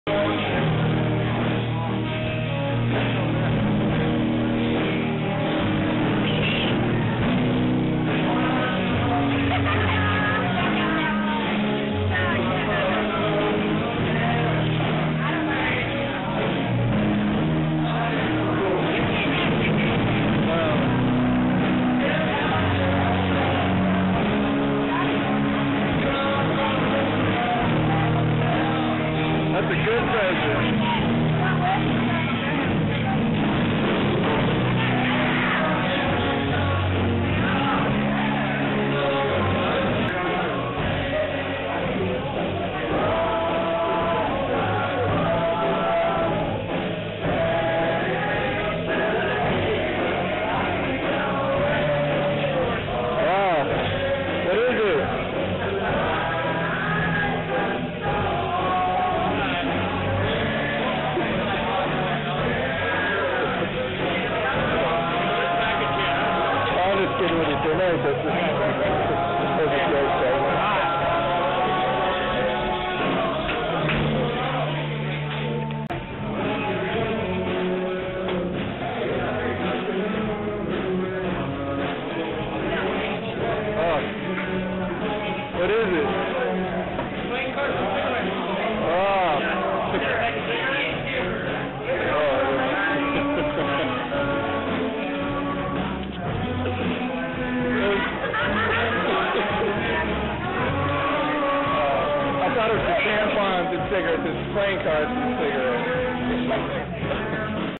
Col. James C. McConville in Iraq, Tampons and cigarettes - Original unedited recovered footage
They told me to wrap the gifts, they said not everyone is going to get something nice, don't wrap up just good things, try to even it out, put a good a with a bad. We had so many so poker chips, playing cards, cigarettes and tampons sent to us, it's like all people think we do when we deploy is smoke cigarettes, gamble, and bleed out or vaginas, I did the best I could considering I didn't want to do it.
Gen. James C. McConville serves as the 40th and current chief of staff of the Army.